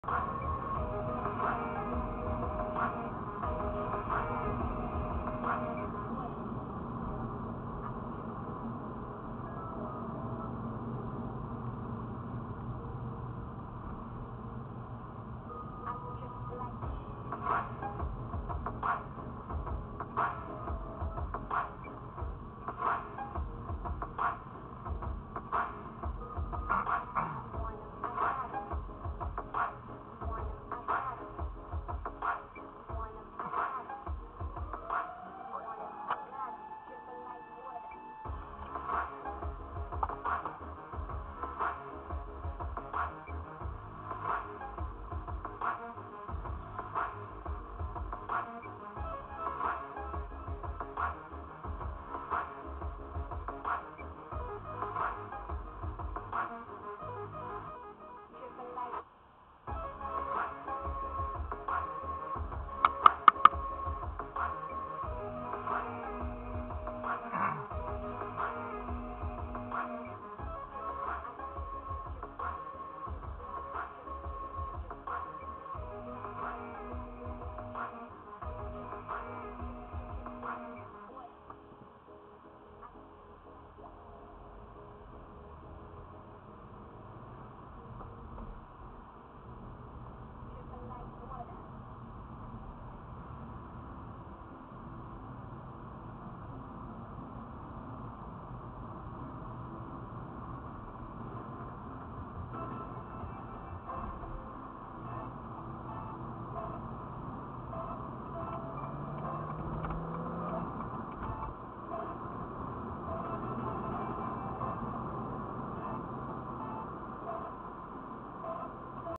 Driver turns on hazard lights and drives 19 in a 35 zone
A Corolla driver, who was riding her brakes down a hill, consistently drives 19 miles per hour in a 35 zone.  She turns on her hazard lights while stopped at a red light at [0:42], leaves them on and continues to drive slow.  I finally pass at [1:50].